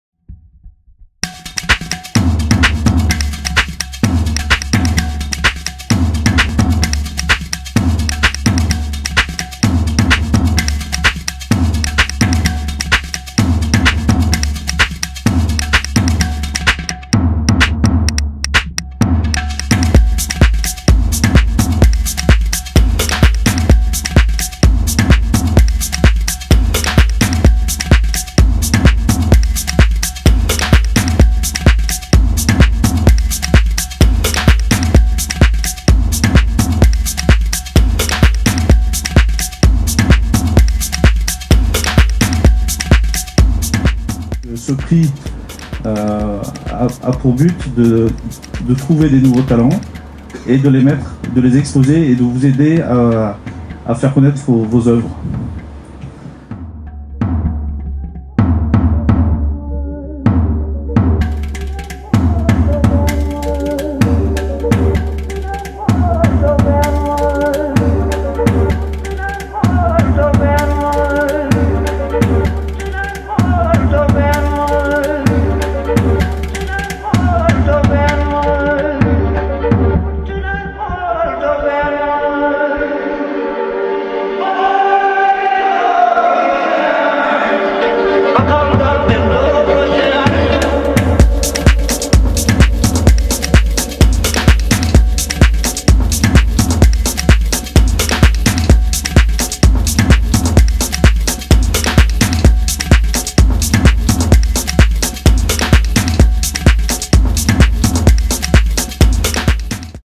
Reportage du Prix Art School 2011
Reportage en musique du Prix Art School 2011 au Batofar à Paris.